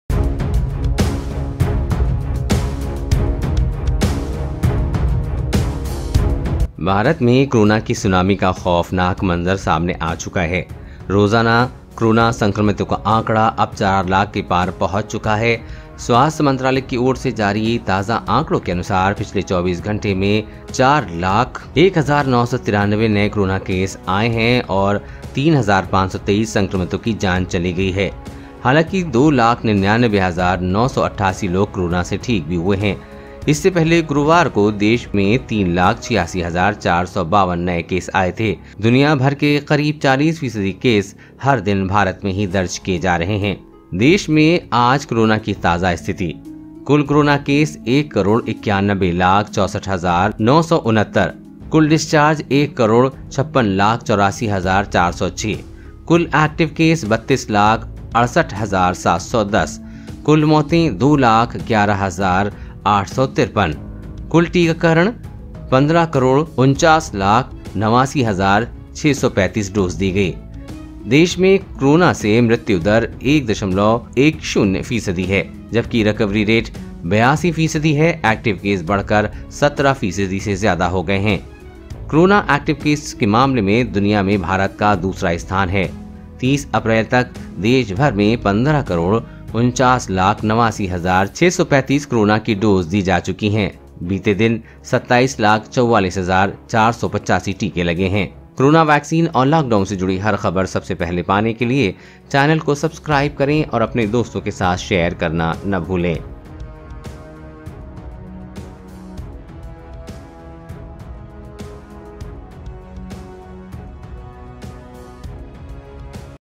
आज भारत में Coronavirus के सामने आये इतने मामले | Coronavirus Cases Updates !
Coronavirus Vaccine : India Records Over 4,01,993 Fresh COVID-19 Cases In Last 24 Hours | Corona Vaccine Latest News & Updates